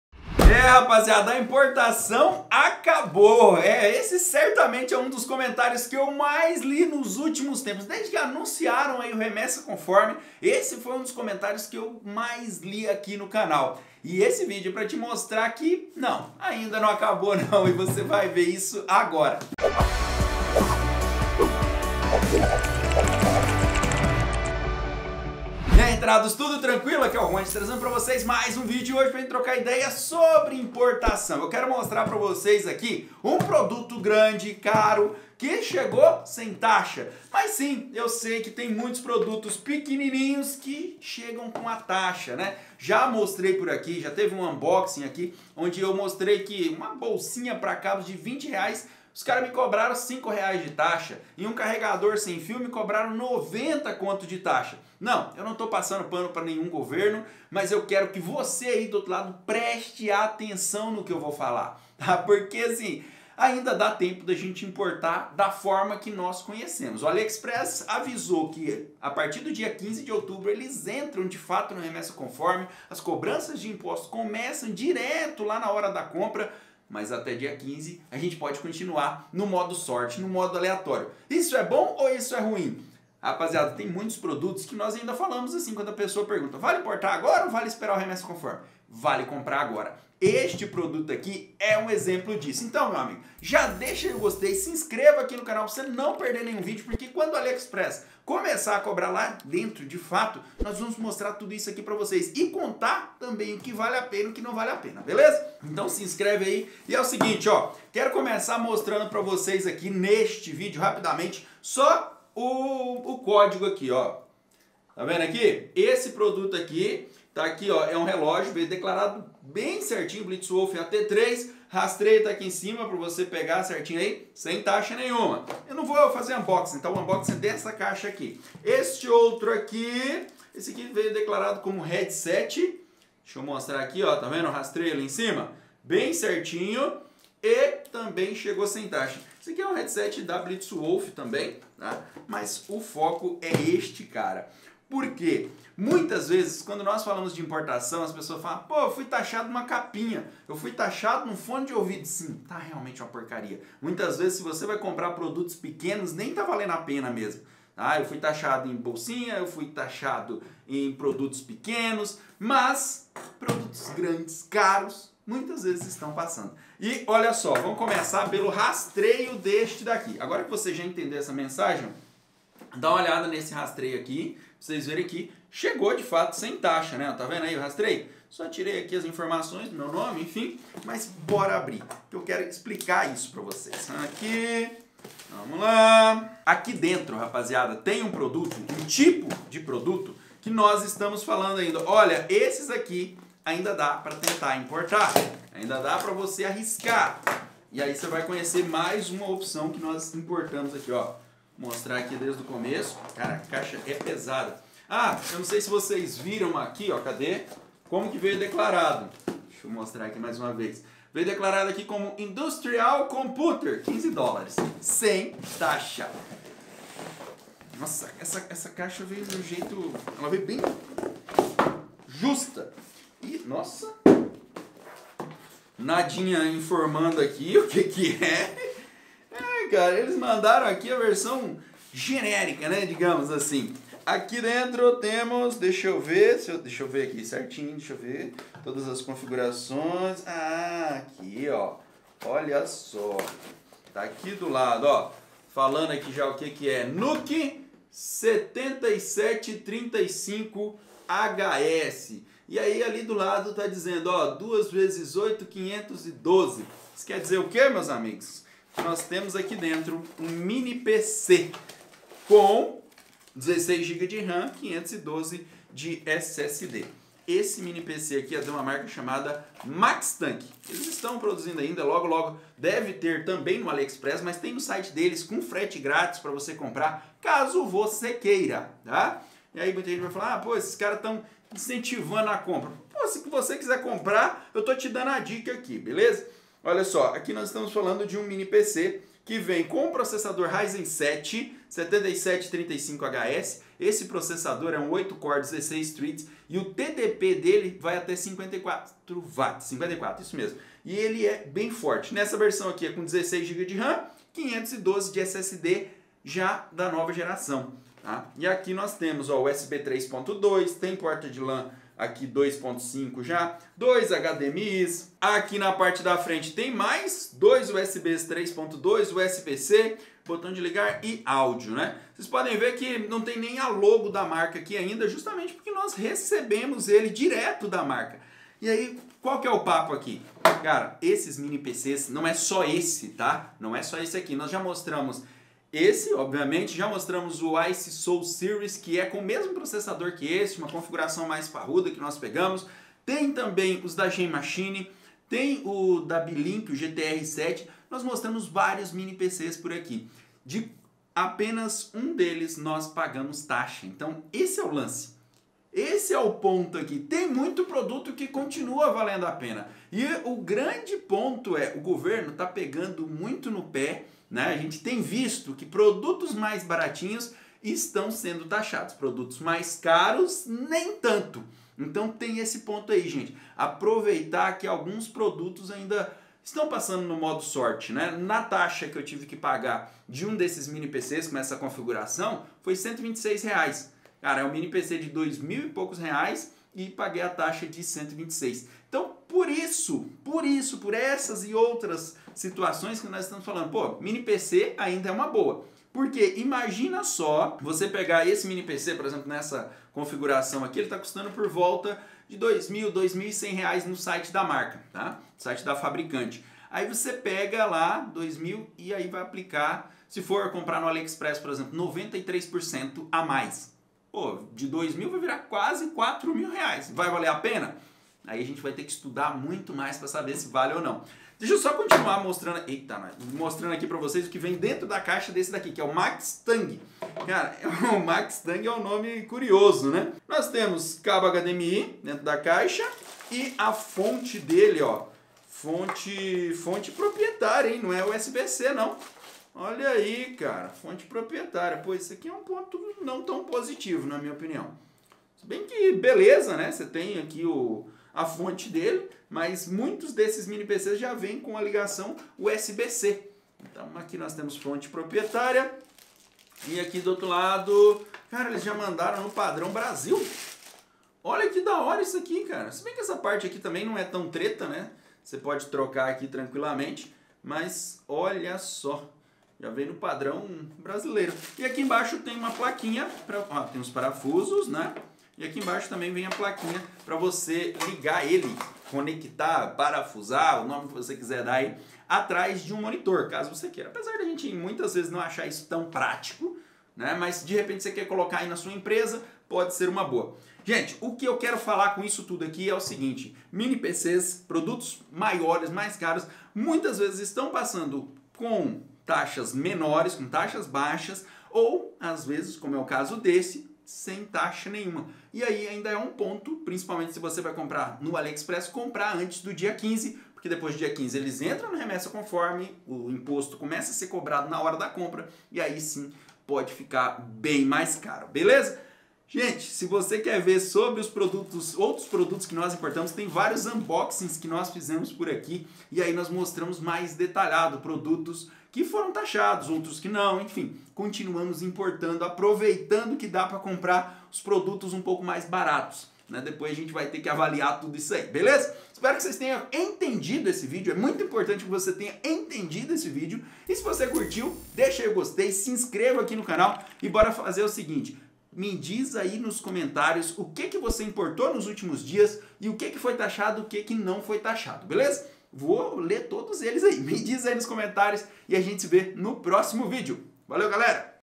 A IMPORTAÇÃO ACABOU? PARA ALGUNS PRODUTOS, NÃO! UNBOXING MINI PC MAXTANG MTN-FP750 (7735HS)
OUTROS ITENS QUE AINDA ACHAMOS BOA IDEIA IMPORTAR:
SSD NVME
Mouse Thunderobot ML903
Power Bank Baseus 65W com 20000mAh